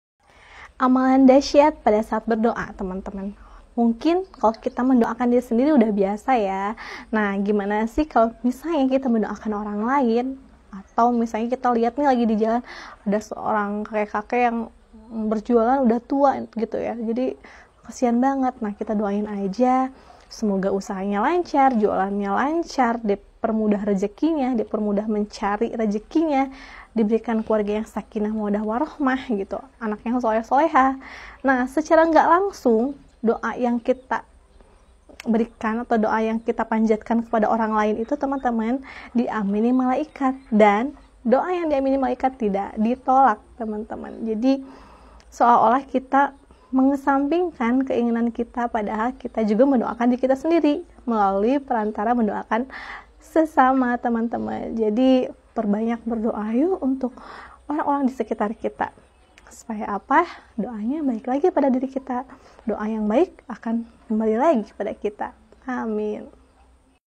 MENDOAKAN ORANG LAIN, DOA KITA DIAMINI MALAIKAT
Mendoakan orang lain diamini malaikat dan doanya baik lagi kepada yang mendoakan . perbanyak mendoakan orang lain yang baik-baik yuu

Mendoakan orang lain, cara doa cepat terkabul, cara didoakan malaikat, doa yang dikabulkan, hajat yang dikabulkan, doa mudah dikabulkan